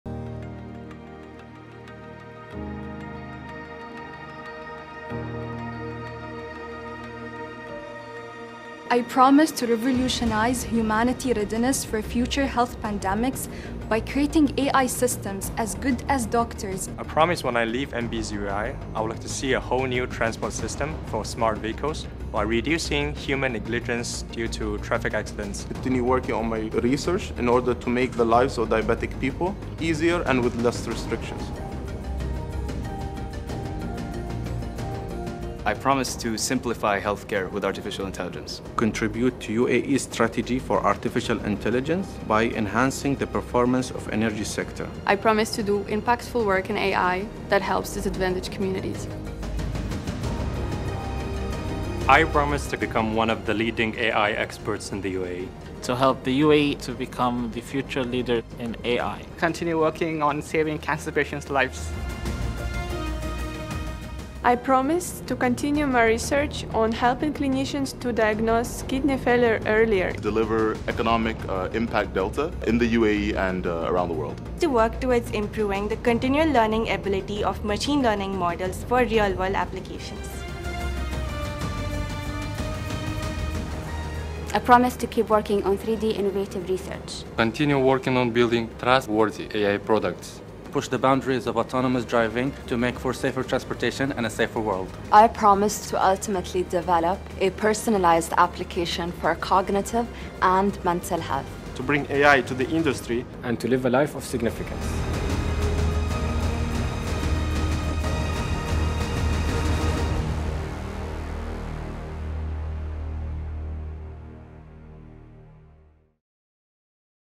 The Promise of MBZUAI Graduates
Our inaugural MBZUAI graduates share the impact they promise to have on the world through continuing their research, applying their new skills in industry, and helping humanity push the boundaries.